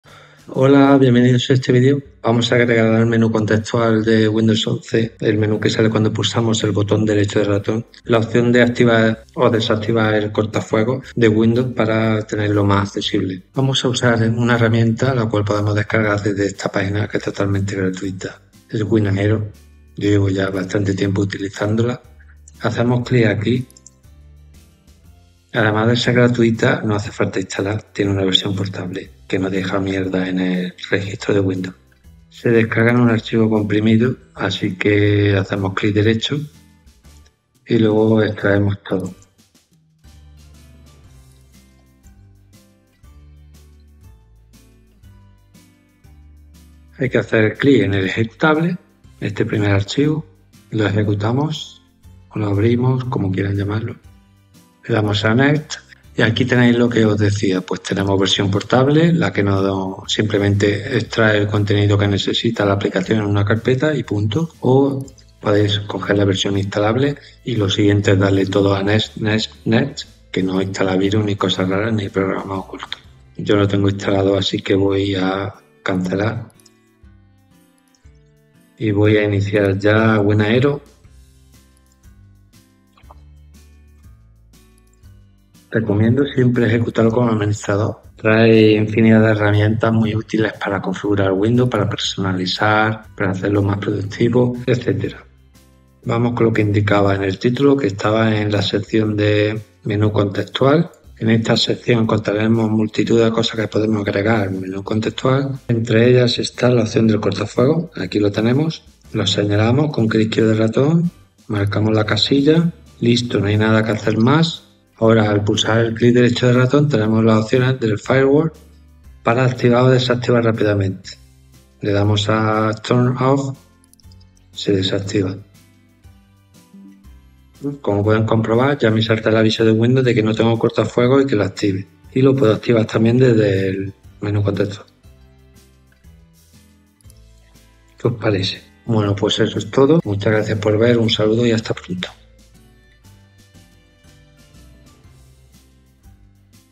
El truco para activar el cortafuegos más rápido y fácil
Videotutorial en el cual enseño mi truco para poder quitar o poner el cortafuegos de windows11 en pocos segundos. El truco es añadirlo al menú contextual (menú del clic derecho de ratón) y así en pocos clic configurarlo a medida.